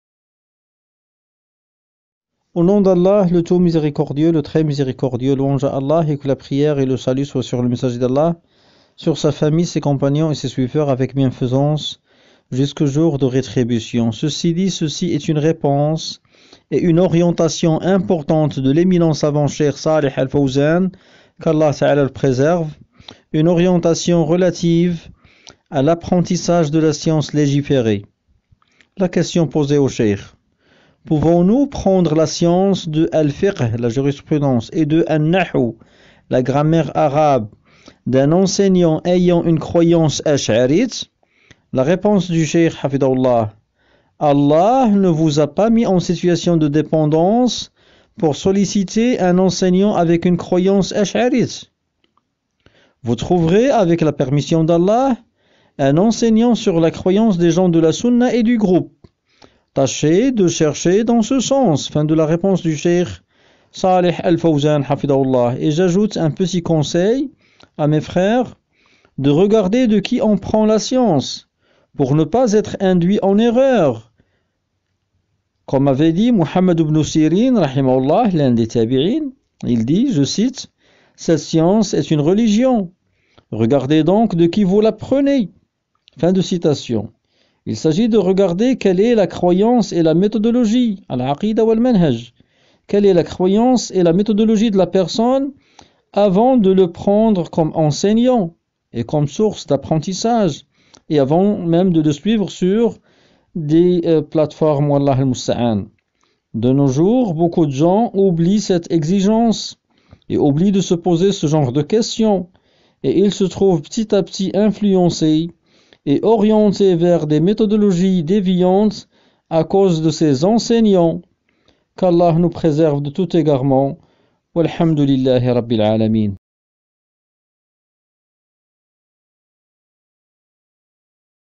Apprendre la jurisprudence ou la grammaire chez un enseignant ach'arite❓
L'érudit savant shaykh Dr. Saleh AlFawzan -qu'Allah le préserve.